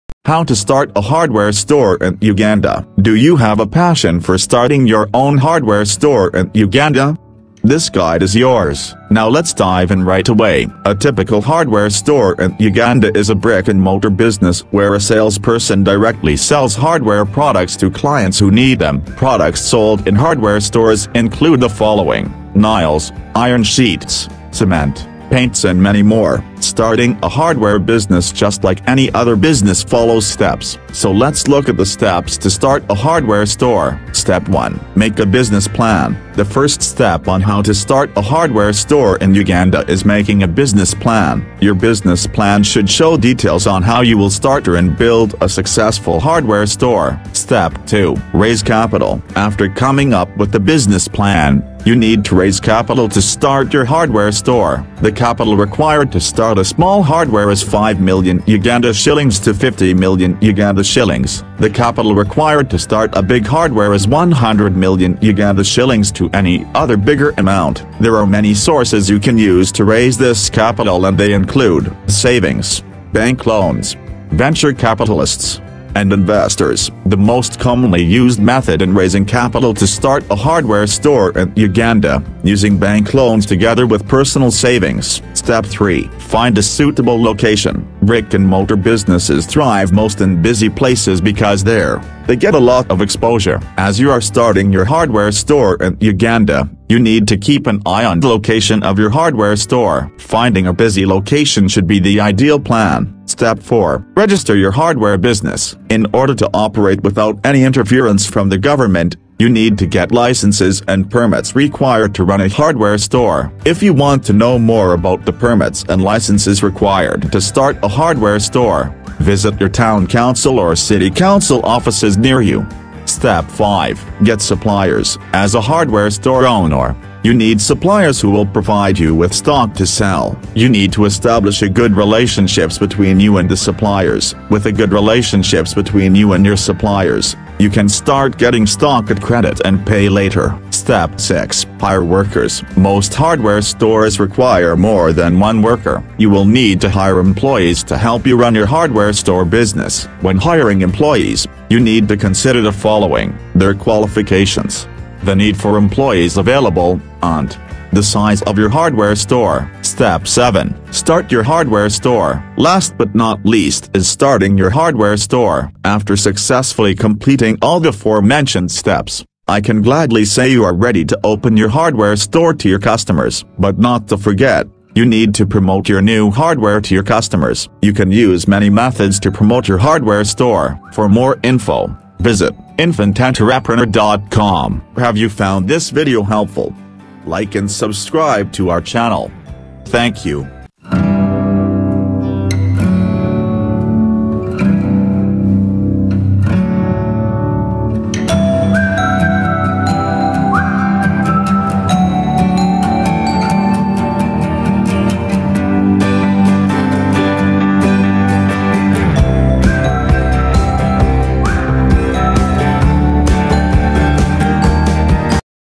How To Start A Hardware Store in Uganda
How to start a hardware store business in Uganda. Here are the steps to follow when starting a hardware business in Uganda.

For more information on starting a hardware business in Uganda, we have a full post.